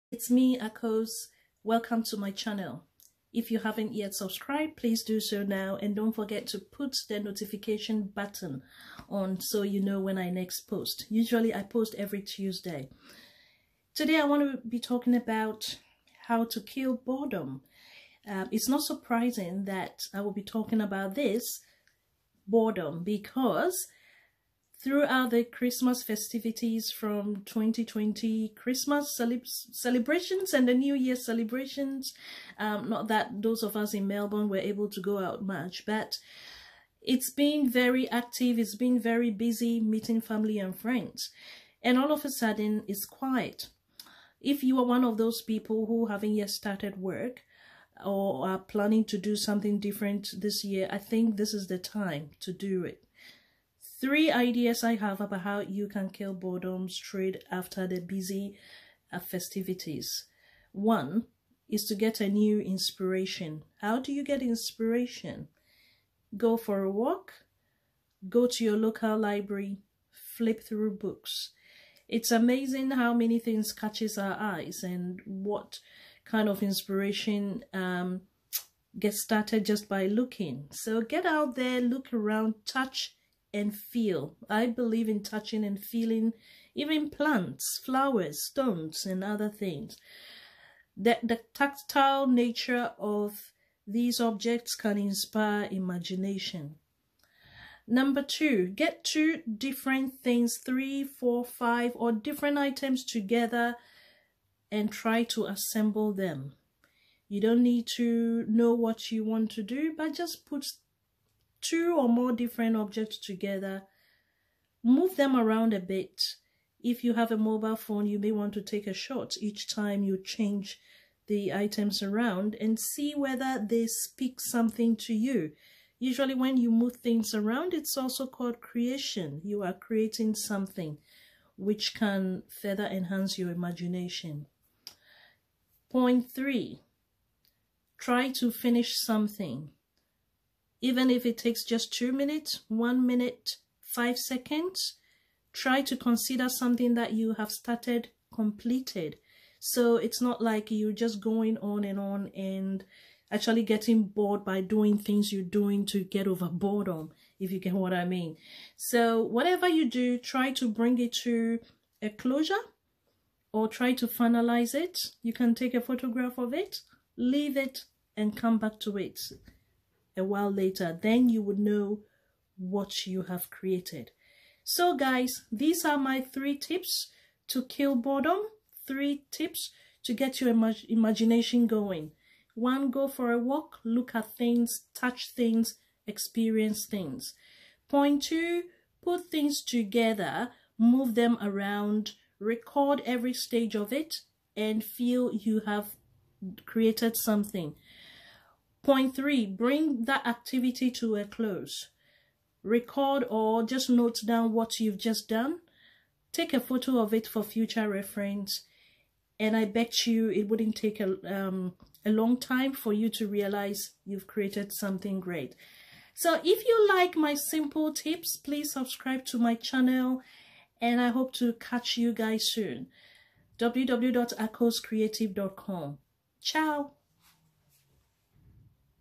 Feeling Empowered towards My 2021 Goals
A week into the New Year and following the busy  Festive season, some start losing focus, boredom and loss of energy sets in. Re calibrate that creative mentality and get back on track. 🏜